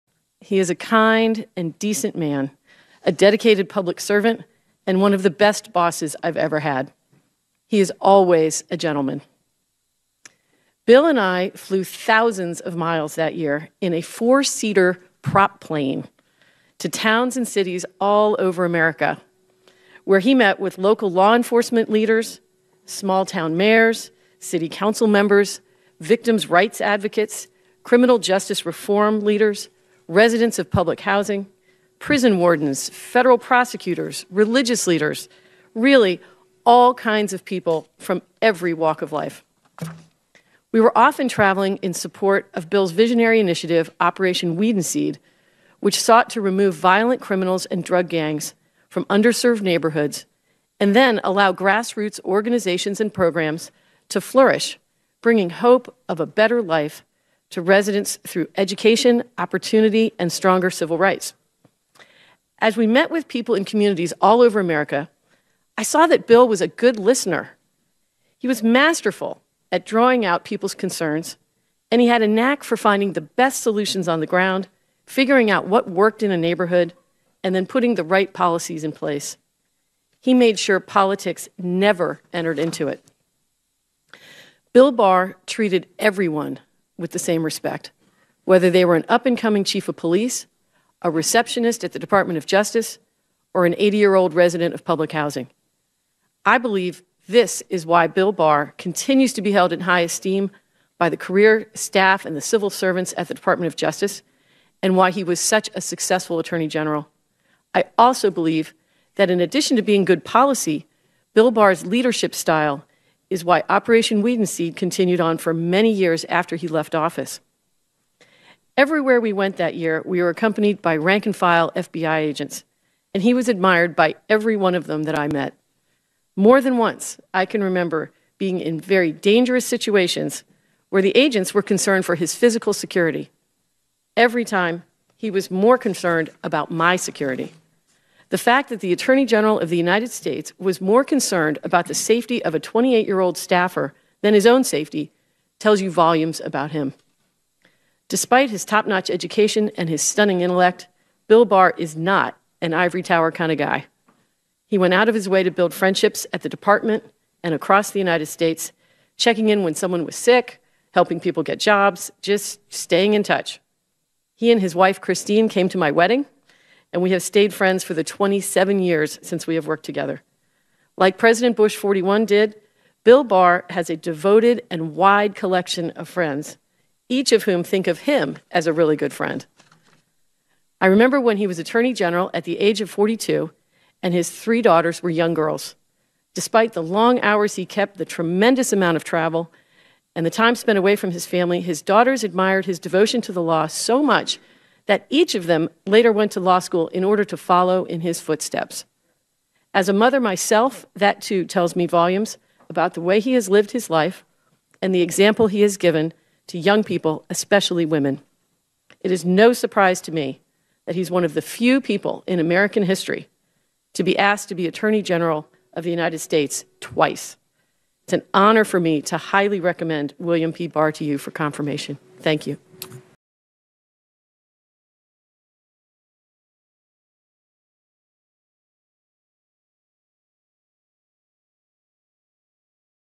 Former Colleague Praises Barr's Leadership, Character, And Devotion To The Law Throughout His Career
Mary Kate Cary, former speechwriter for President George H.W. Bush and longtime friend of former AG William Barr, praised Barr’s leadership, character, and devotion to the law in her testimony before the Senate Judiciary Committee on 01/16/2019.